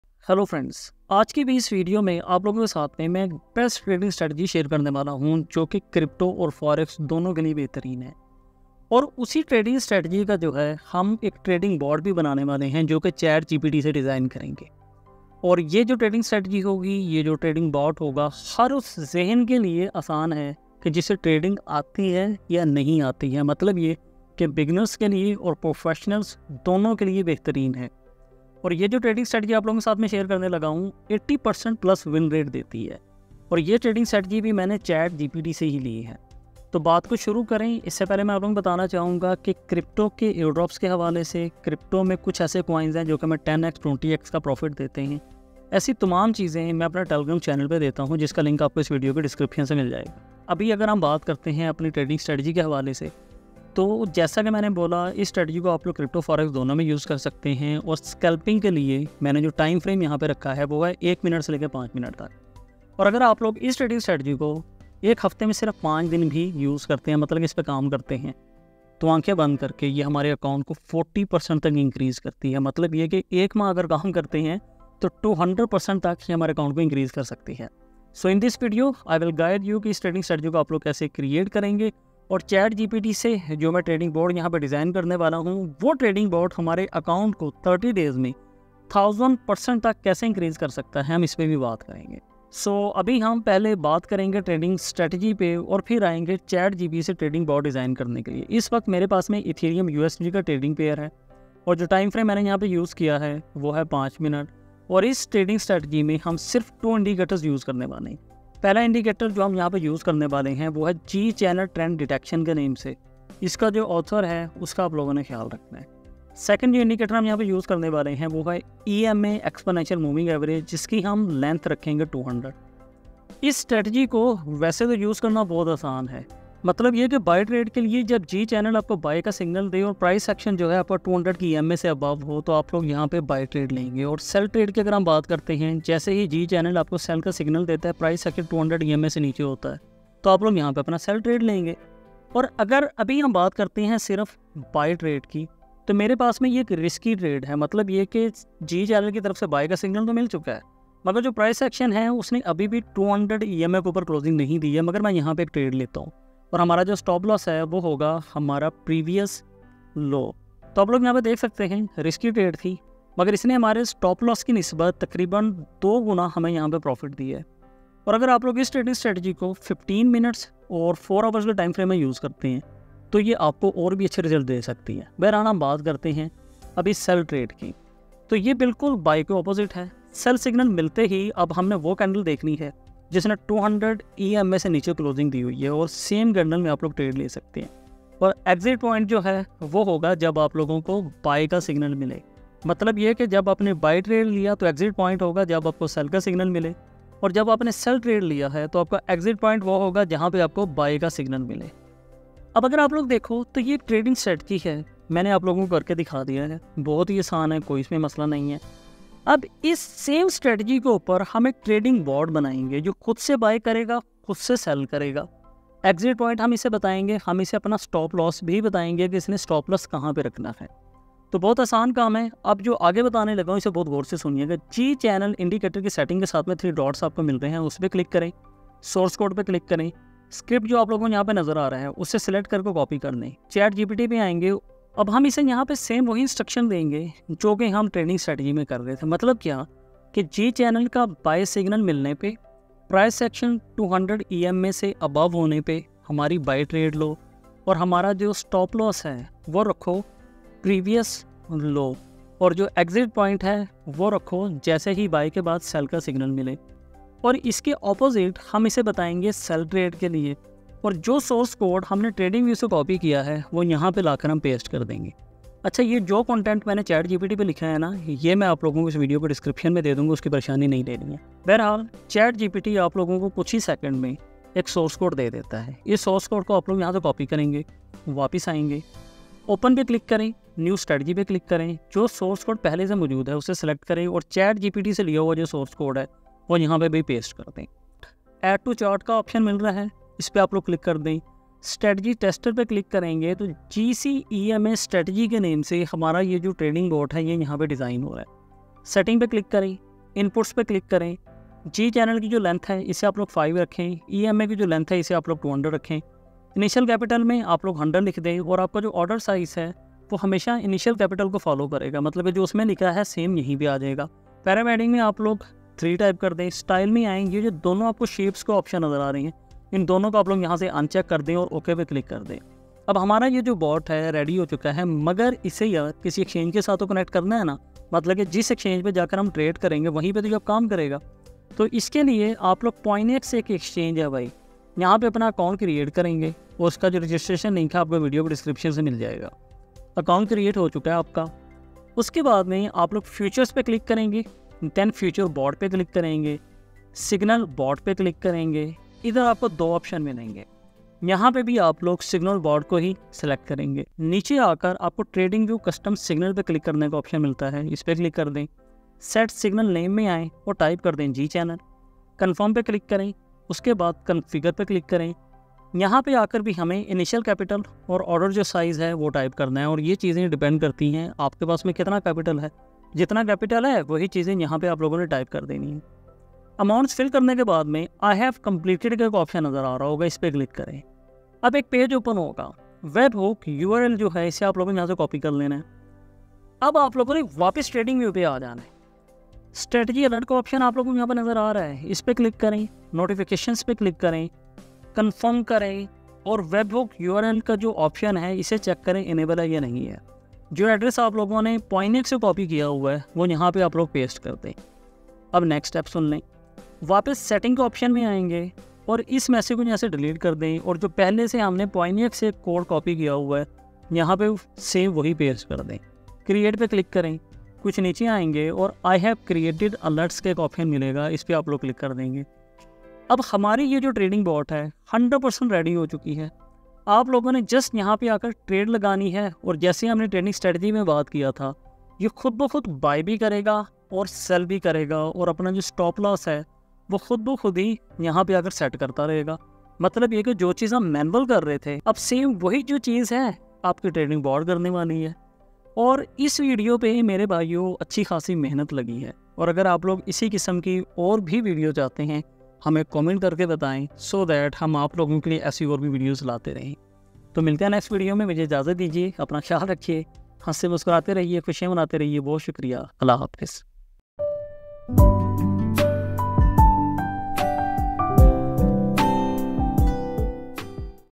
💰Make Your Own Bot | Mastering Forex and Crypto Trading Strategy with G-Channel and EMA 200 🚀🚀🚀
Req Text For Bot: Code a simple trading strategy in pinescript  with the following indicators - G-Channel and EMA.
Buy When 1) G-channel gives a buy signal; 2) price is closed above 200 EMA and stop loss candle will be previous candle closed below 200ema.

Sell When 2) G-channel gives a sell signal; 2) price is closed below 200 EMA and stop loss candle will be previous candle closed above 200ema. the indicator source code -

💁‍♂️ In this power-packed video, unlock the secrets of a highly effective trading strategy tailor-made for both Forex and cryptocurrency markets. Join us as we delve into the intricacies of utilizing just two indicators: G-channel trend detection and Exponential Moving Average 200. Learn how to spot lucrative opportunities with precision using our buy and sell setups, backed by comprehensive explanations and real-world examples.

Key Features:

Discover the G-channel buy signal strategy paired with price action analysis above the EMA 200 for optimal entry points.
Uncover the secrets of setting stop-loss levels based on previous highs to mitigate risks effectively.
Explore the flip side with sell setups, ensuring you're equipped to capitalize on market downturns.
Dive deep into the world of automated trading with a step-by-step tutorial on creating a trading bot using ChatGPT technology.
Witness the magic as we generate source code live and seamlessly integrate it into TradingView, with Pionex as our preferred exchange platform.

Get ready to revolutionize your trading game and maximize your profits with expert insights and actionable strategies. Don't miss out on this golden opportunity to elevate your trading journey to new heights!